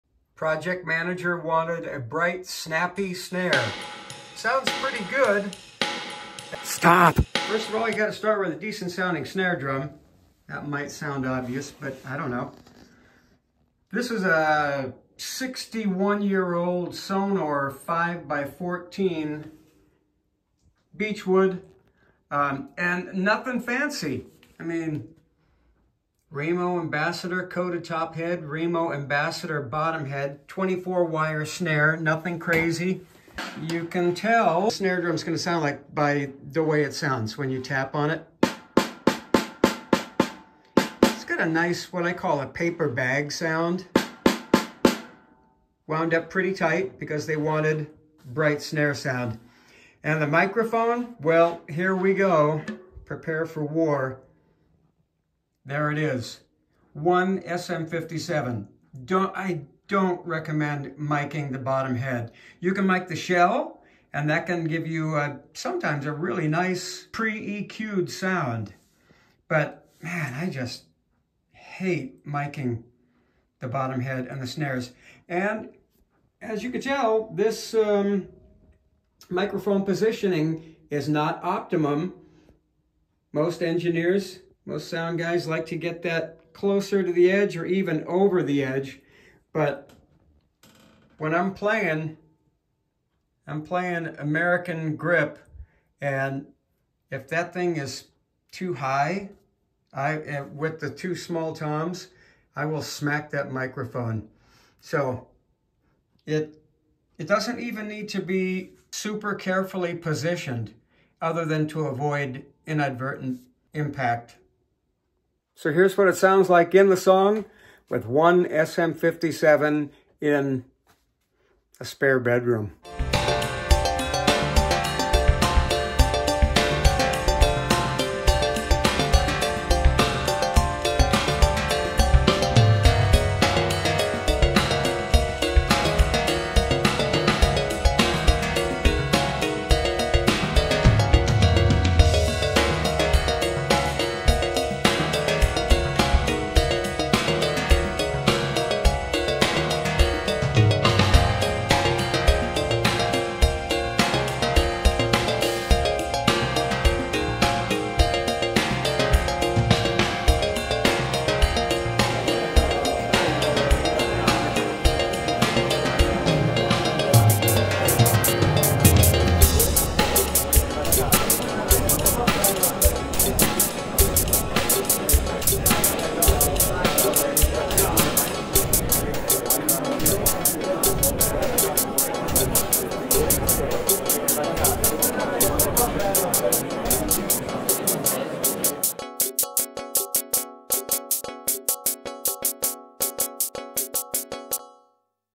One snare mic is all you need!!
It's true! One mic, and it doesn't even have to be a special, unusual, snare only mic. Almost anything will do. I use a Shure SM57. The results are excellent. I DO NOT recommend micing the bottom head or snares. I seriously don't understand why this is standard practice. It just doesn't work for me. Maybe I need a bigger room.